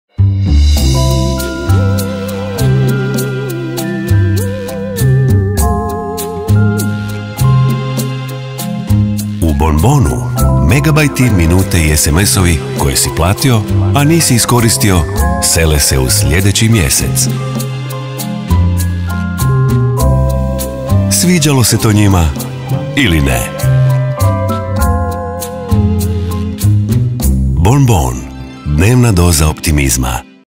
U bonbonu se tvoje neiskorištene jedinice sele u sljedeći mjesec
U bonbonu, MB, min i SMS-ovi koje si platio, a nisi iskoristio,
sele se u sljedeći mjesec.
Sviđalo se to njima ili ne.
bonbon. Dnevna doza optimizma.